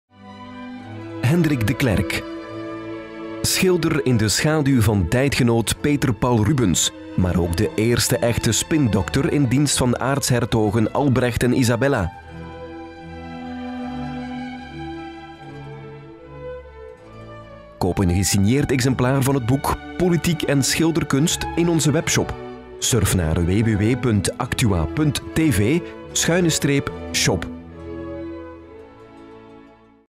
Promo: Politiek & schilderkunst - Katharina Van Cauteren
Surf naar om ons uitgebreid boekenaanbod te ontdekken.

Auteur(s): Katharina Van Cauteren
Pagina’s: 416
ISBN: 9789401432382
2015

Op een handvol kunsthistorici na heeft geen gezond mens ooit al gehoord van de Brusselse schilder Hendrick De Clerck (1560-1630). Nochtans was De Clerck een tijdgenoot van Peter Paul Rubens en werkte ook hij voor de aartshertogen Albrecht en Isabella. Maar waar de grote Peter Paul de geschiedenis in ging als artistieke trendsetter en schilderende krachtpatser, werd Hendrick De Clerck roemloos vergeten.

Toch zijn de fijnzinnige en kleurrijke taferelen die De Clerck schildert voor Albrecht en Isabella politieke propaganda van het hoogste niveau. En daarin is the sky the limit: de infante Isabella wordt er opgevoerd als wijze Minerva, kuise Diana of Maagd Maria. Dat is nog niets in vergelijking met haar echtgenoot: op De Clercks schilderijen verandert Albrecht in de zonnegod Apollo, of in Jezus Christus zelf.

Slimme beeldstrategieën maken Hendrick De Clerck zo tot een vergeten hoofdrolspeler in één van de meest ambitieuze projecten die de geschiedenis ooit heeft gezien. Voor wie weet hoe ze te lezen, vertellen zijn schilderijen een verhaal van macht, politieke beloftes, en een ietwat gezwollen zelfbeeld. Want ook als vorst ben je slechts zo belangrijk als je jezelf maakt.

Prof. dr. Katharina Van Cauteren (1981) is als kunsthistorica gespecialiseerd in de vroegmoderne periode. Met meeslepend enthousiasme en de nodige zelfrelativering neemt ze de lezer mee naar een vervlogen wereld. Zo wekt ze de schilderijen en hun opdrachtgevers weer tot leven. Het resultaat is soms grappig, soms tragisch, en vaak ook verrassend actueel.